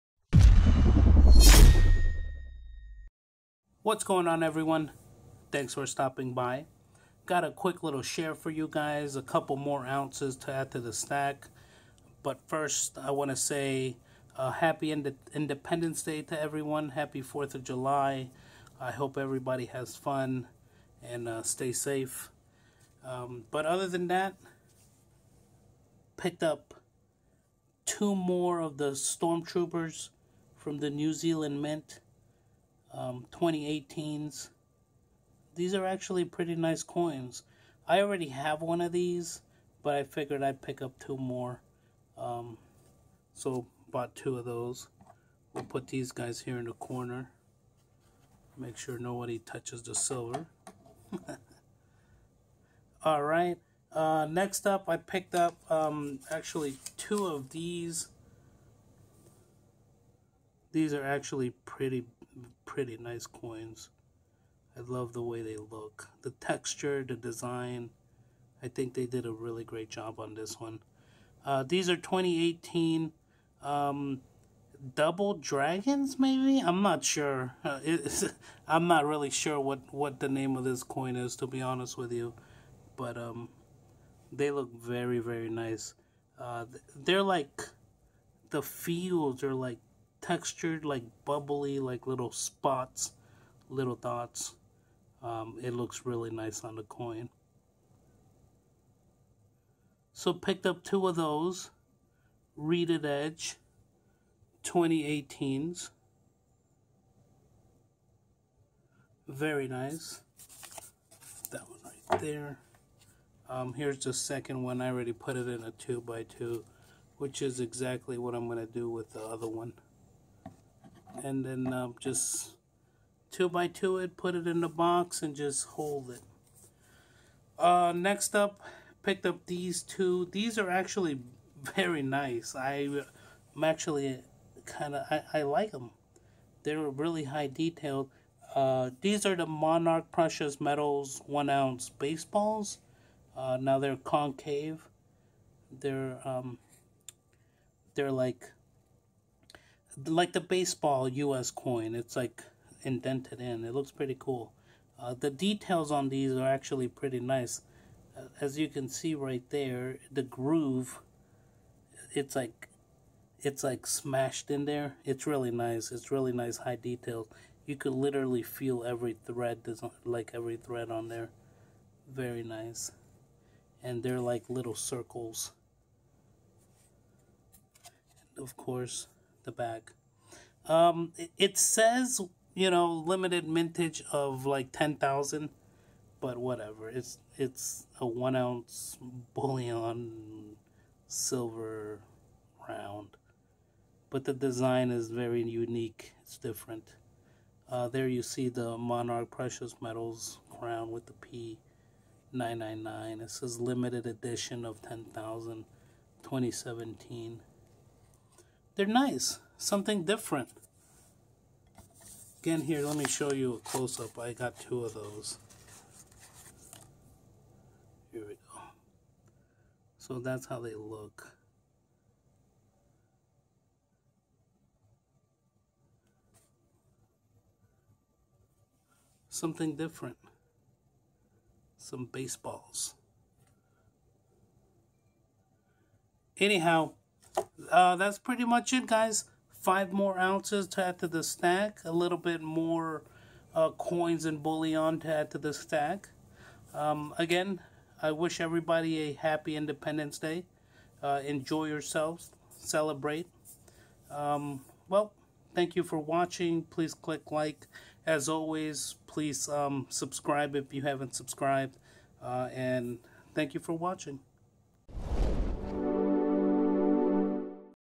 2018 Double Dragons, Storm Troopers Silver Coins, Monarch Baseball Rounds
232/400... Up to 232 oz of Silver for 2018. New additions to the horde/stack. My journey to 400 oz continues.